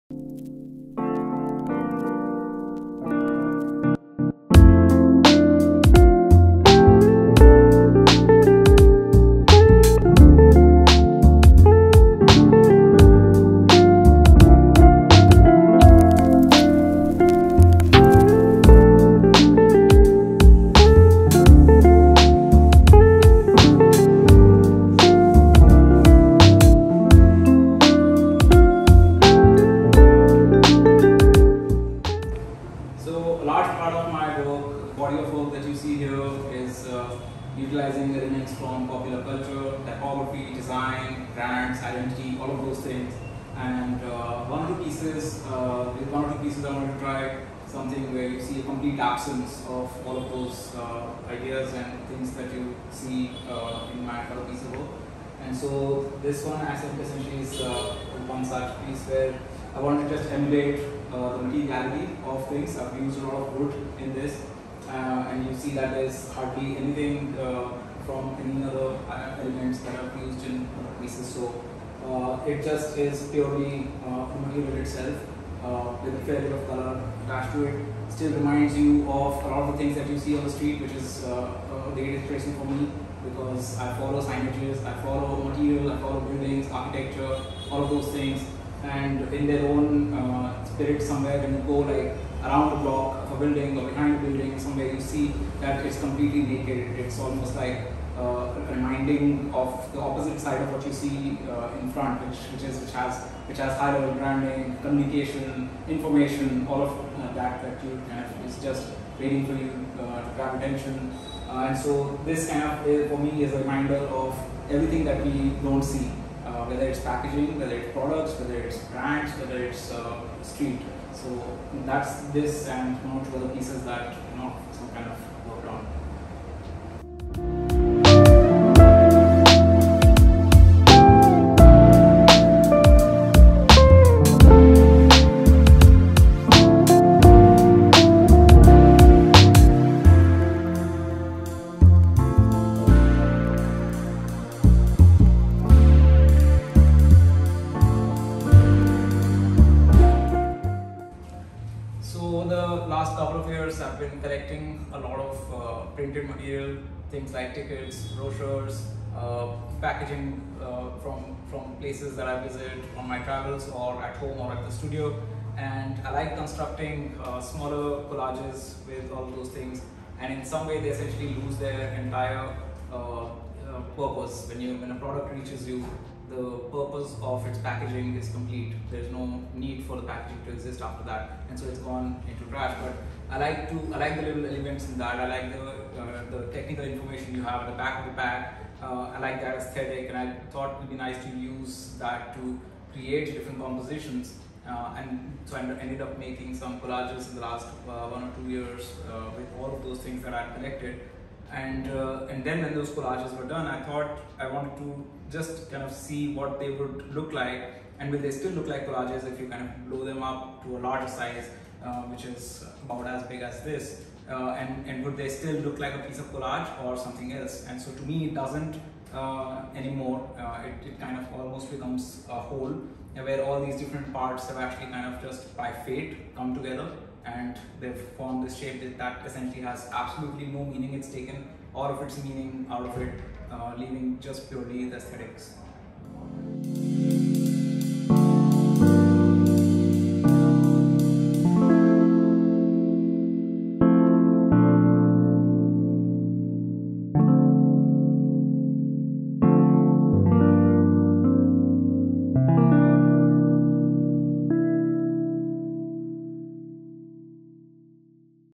Information Architecture by Kunel Gaur
Presented by Method Art Space, Information Architecture™ is a collection of contemplative abstract sculptural works and bricolage that play upon the absence of function whilst still using all kinds of functional aesthetics, and hardware, in the way that the only purpose they are left with is to exist. Gaur has used architectural materials like wood, concrete, resin, metal, acrylic paint and glass in this work.

Video: Courtesy of Method Art Space